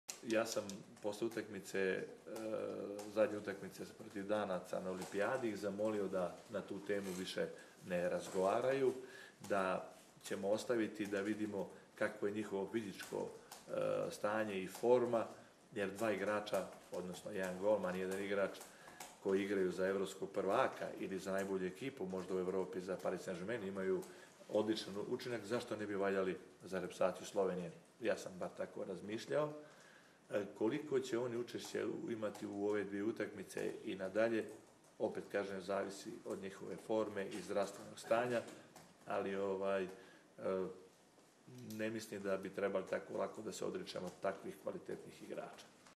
VESELIN VUJOVIĆ
Selektor SLO reprezentance Veselin Vujović o ponovnem vpoklicu Uroša Zormana in Blaža škofa v reprezentanco.